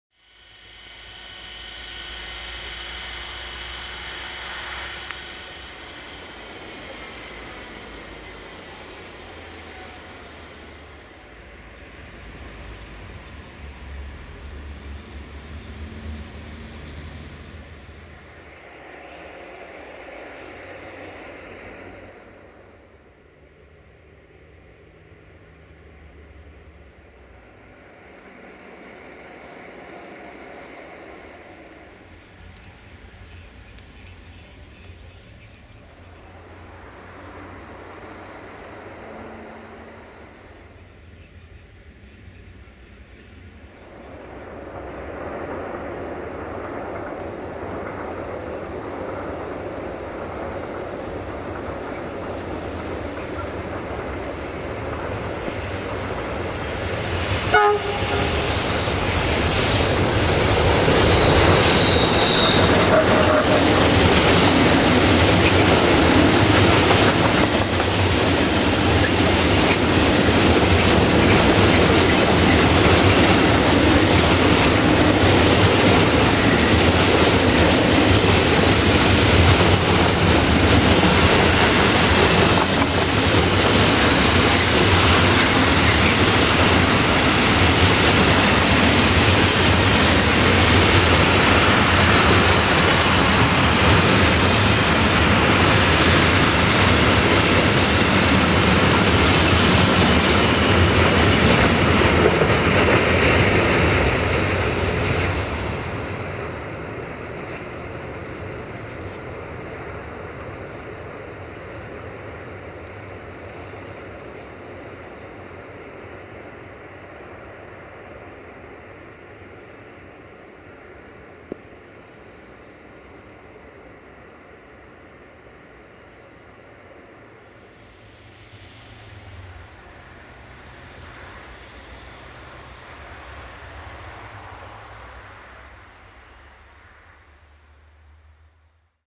LDE2100 609 011-5 & Marfar DB Cargo Freight Train in Oradea [Autumn Edition] - 04 October 2018
1:DB Cargo Romania Freight Train/Marfar/Guterzug - 060-DA/LDE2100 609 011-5 RO-DBC

[Autumn Edition]

04 October 2018